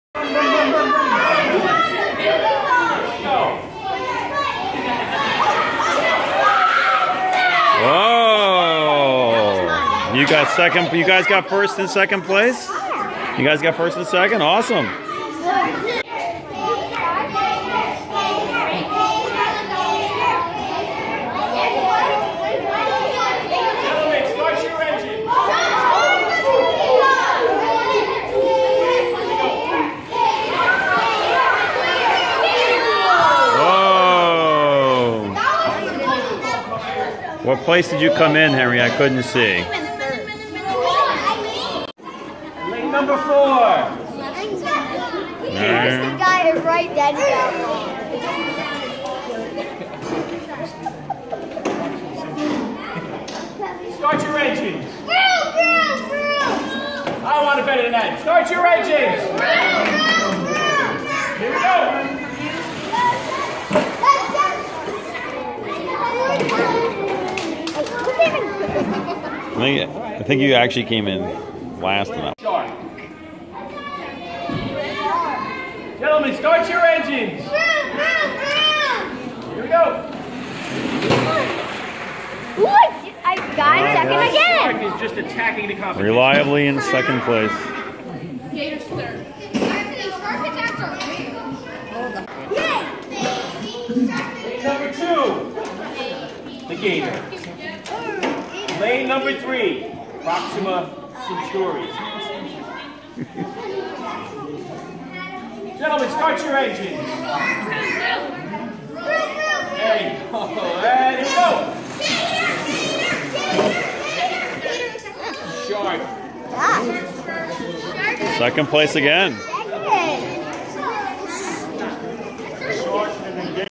Pinewood Derby 2019
Cub Scout Pack 102 in the Bronx does Pinewood Derby!